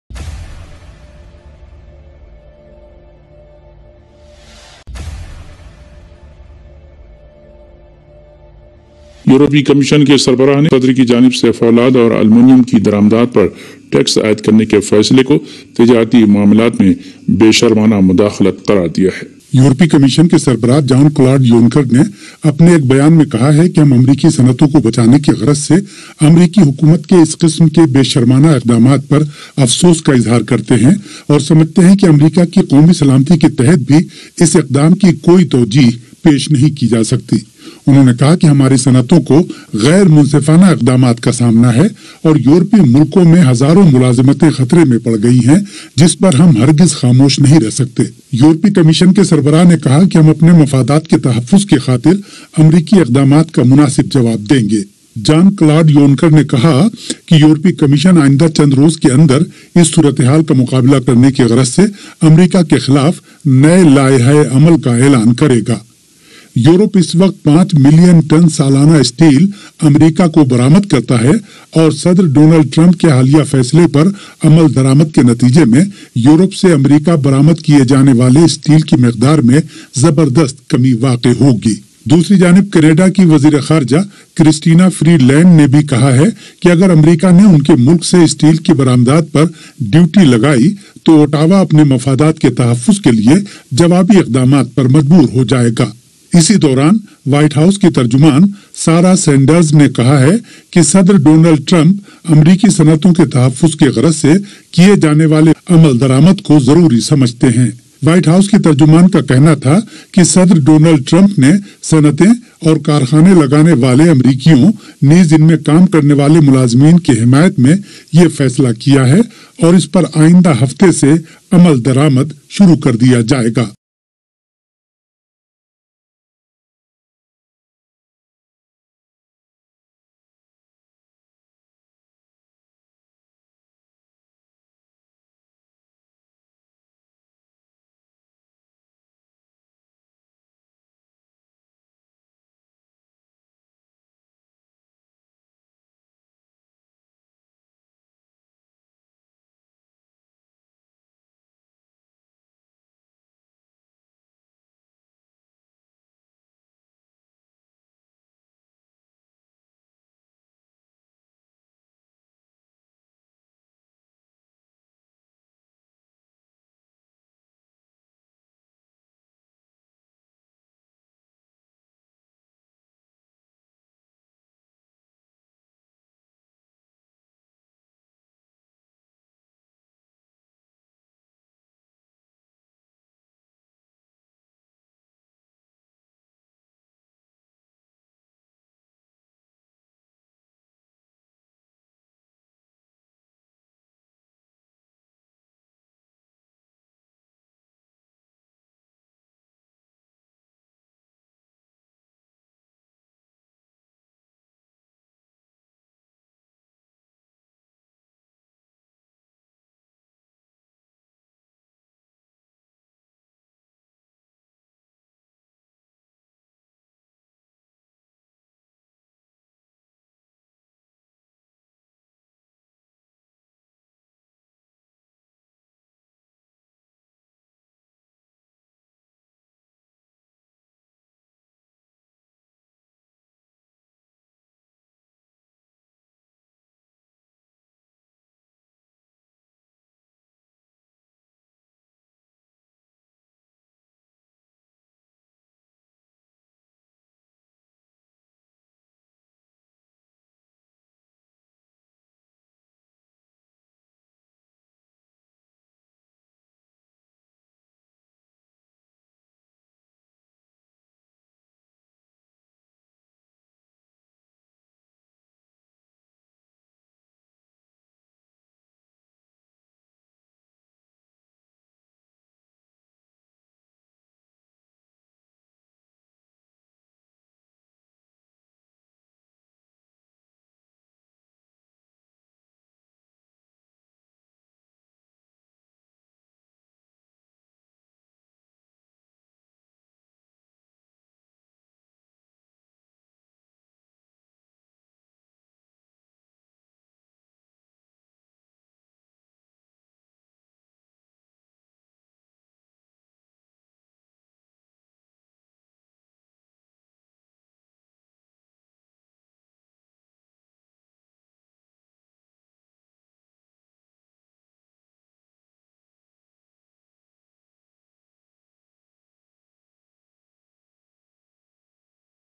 American duty increase on European steel
یورپی یونین نے امریکی صدر کی جانب سے اسٹیل کی درآمدی ڈیوٹی میں اضافے کی شدید مذمت کرتے ہوئے کہا ہے کہ امریکا کو یورپی ردعمل کا سامنا کرنے کیلئے تیار رہنا چاہیئے ۔۔۔